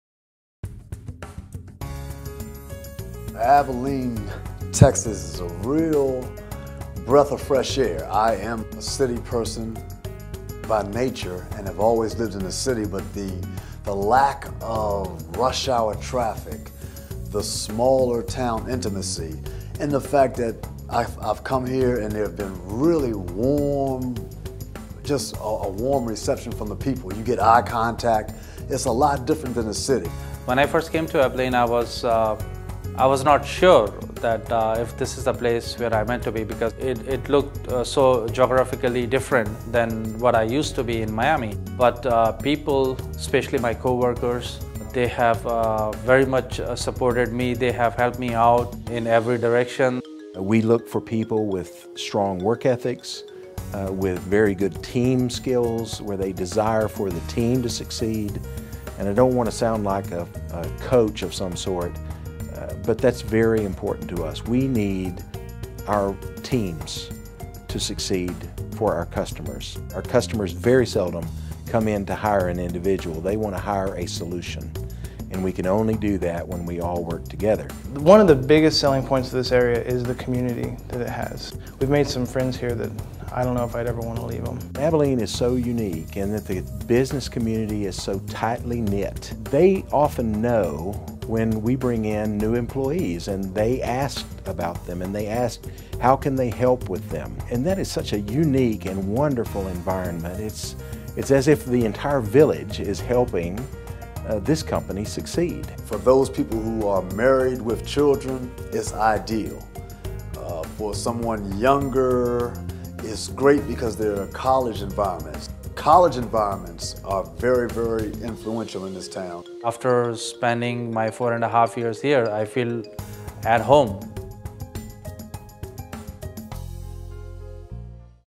Abilene, Texas: A great place to work and live!
Hear firsthand from some of our people the advantages to working at WaveCreste and living in Abilene. Learn more about WaveCreste, our career opportunities, and our Rural-Sourced solutions

TRANSCRIPT: "Abilene, Texas, is a real breath of fresh air. I am a city person by nature and have always lived in the city, but the lack of rush hour traffic, the smaller town intimacy, and the fact that I've come here and they've been really warm, just a warm reception from the people. You get eye contact; it's a lot different than the city.

When I first came to Abilene I was not sure if this is the place I'm meant to be because it looked so geographically different from what I was used to in Miami, but the people, especially my coworkers, they have very much supported me, and they have helped me out in every direction.

We look for people with strong work ethics, with very good  team skills with a desire for the team to succeed. I don't want to sound like a coach of some sort, but that's very important to us. We need our teams to succeed for our customers. Our customers very seldom come in and hire an individual. They want to hire a solution, and we can only do that when we all work together.

One of the biggest selling points of this area is the community that it has. We've made some friends here that I don't know if I'd ever want to leave them.

Abilene is so unique in that the business community is so tightly knit. They often know when we bring in new employees, and they ask about them and they ask how can they help with them. And that is such a unique and wonderful environment. It's as if the entire village is helping this company succeed.

For those people who are married with children, it's ideal. For someone younger, it's great because there are college environments. College environments are very influential in this town.

After spending my four and a half years here, I feel at home."